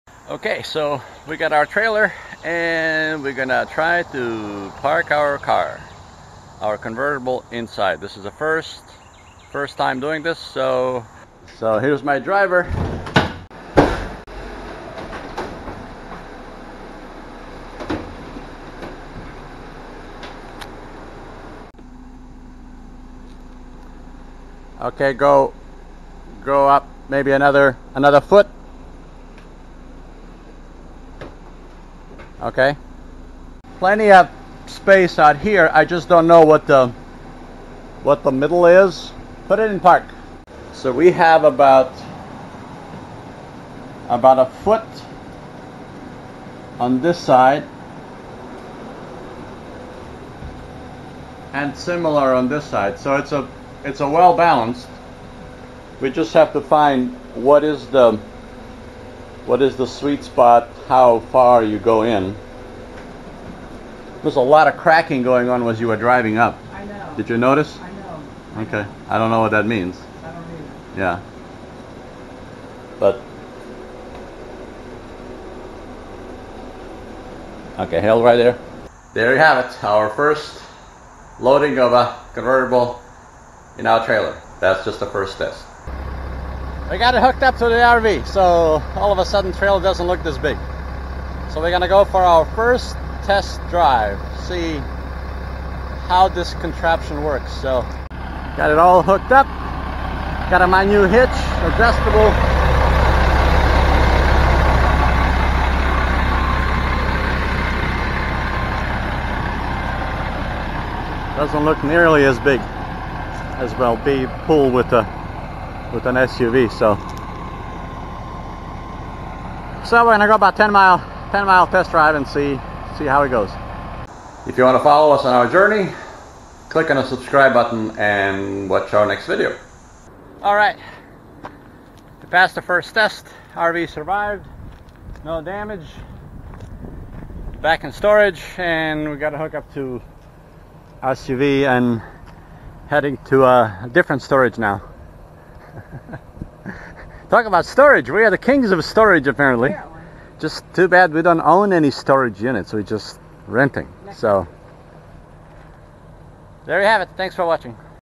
First motorhome pull with car in the trailer
We loaded our convertible into our trailer and went on the first test drive.  When loaded, there is not a lot of space on either side of the car.  Legal width for enclosed trailers is 8.5 feet and our BMW convertible fits tight.  If you have a large SUV or pickup truck, measure first.  When it comes to pulling it, towing the trailer with car inside didn't make any difference to our motorhome.  Sixty thousand lbs pulling 8k is not noticeable but you do feel ( see ) the trailer when turning.  We made it!  Twenty minute drive resulted in no damages or mishaps so we are encouraged.